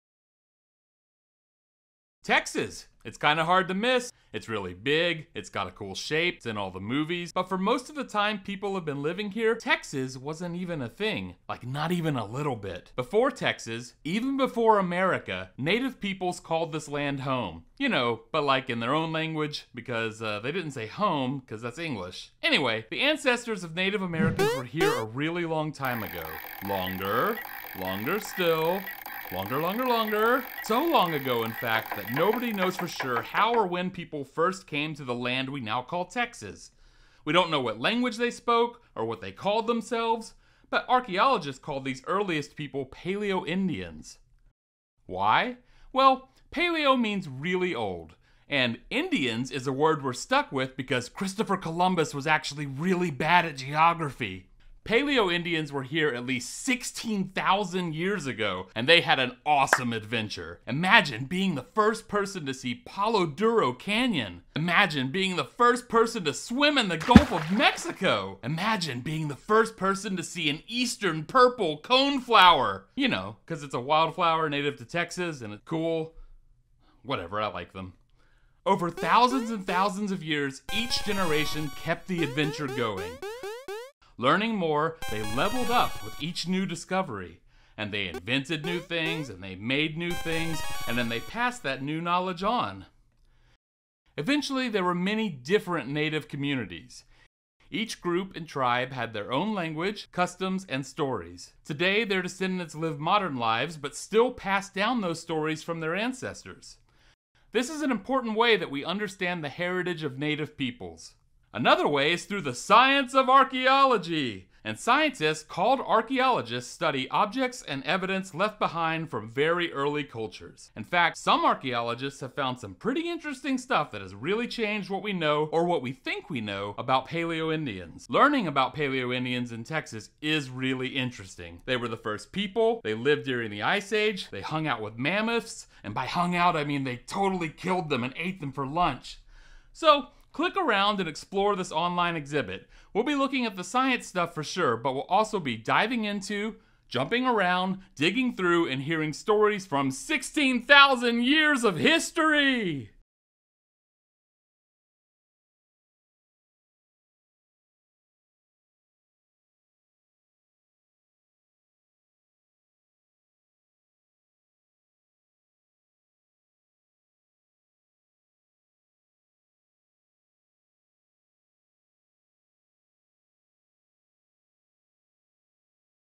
From The Top! - A Time Before Texas
When mammoths, saber-toothed cats, and giant sloths roamed Texas, people were there too.